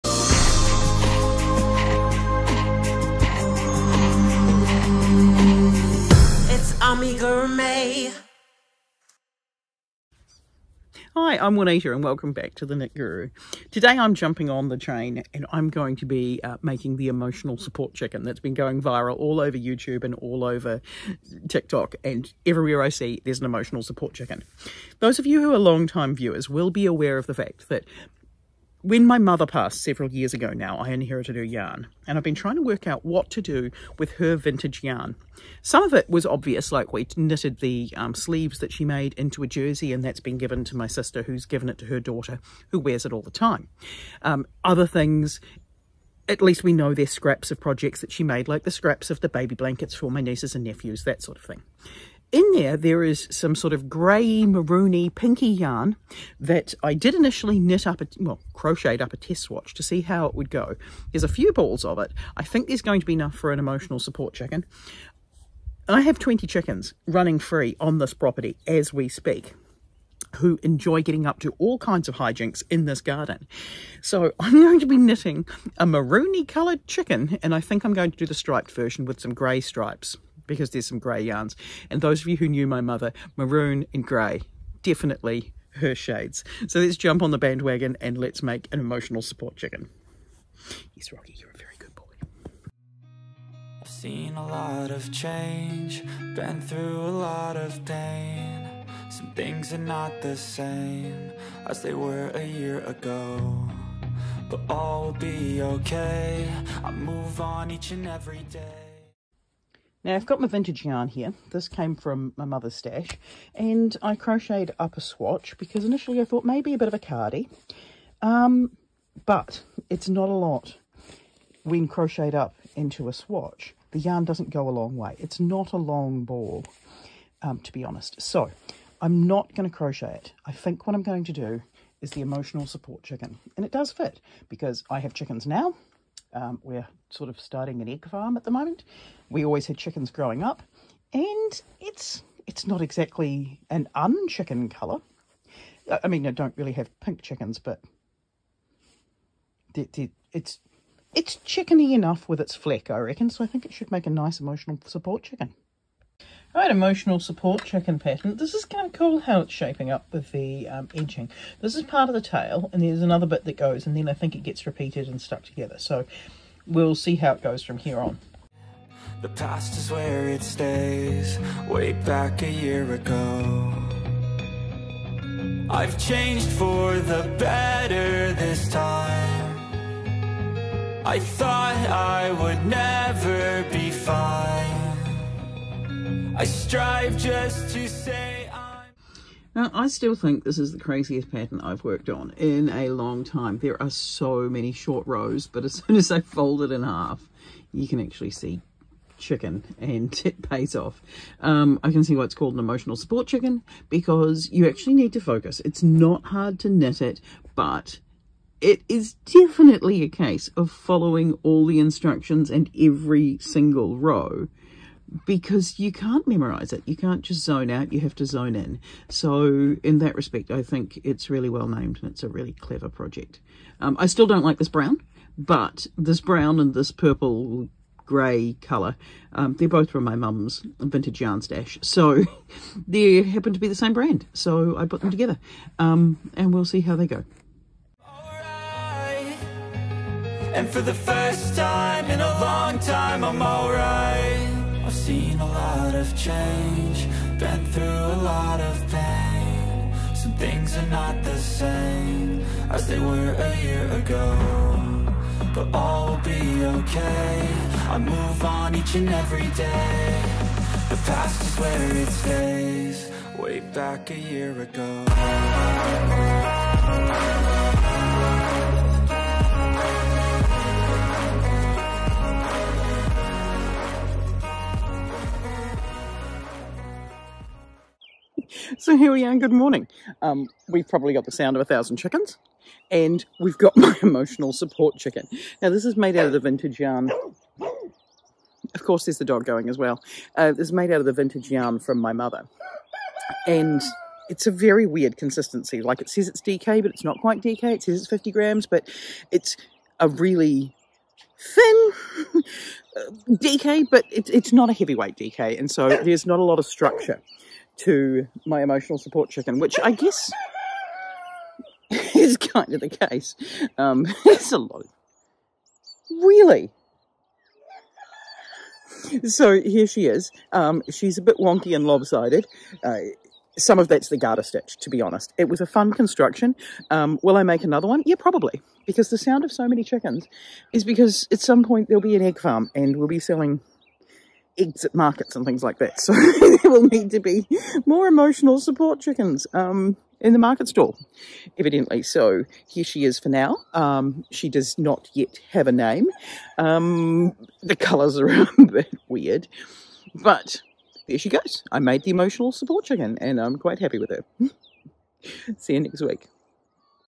Knitting the Emotional Support Chicken - Amigurumi for Amiguru-May
This chicken has been all over my feed, so I jumped on the bandwagon and knitted it.

May is Amigurumi month - Amiguru-may if you will. And I think I am hilarious, this means that I am releasing only Amigurumi related content in May. enjoy.

For free patterns and further knitting advice, visit my I can be found on Ravelry, Ribblr, Instagram, Tik Tok and Etsy as The Knit Guru.